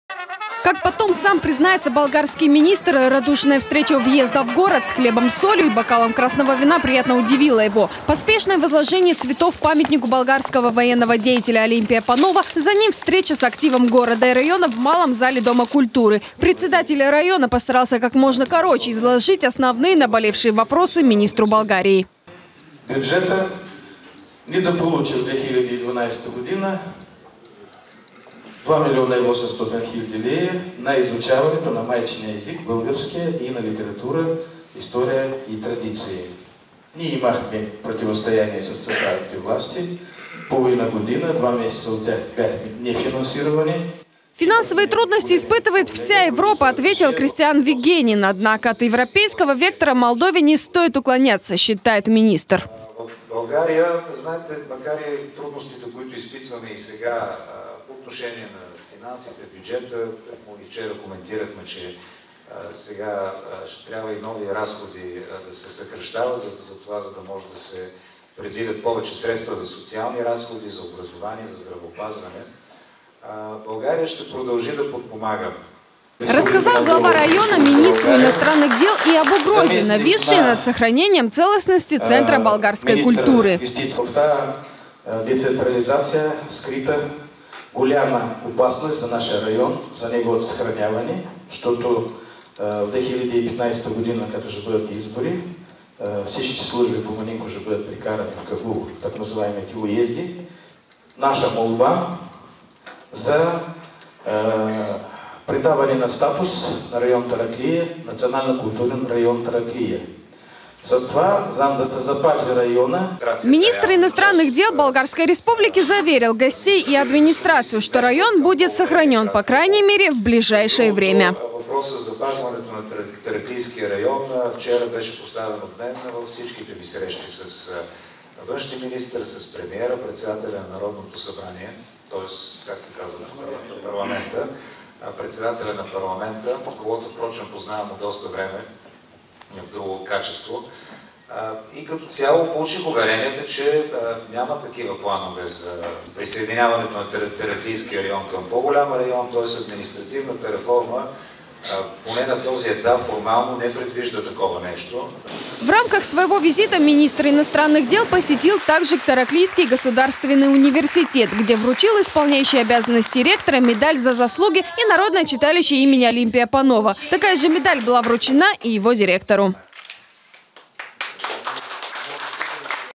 Глава МИД Болгарии в Тараклии
В ходе своего двухдневного визита в Молдову министр иностранных дел Болгарии Кристиан Вигенин посетил и Тараклию. Это был первый визит в новоиспеченного чиновника в центр болгарской культуры нашей республики. Кристиан Вигенин занял пост министра иностранных дел в мае этого года.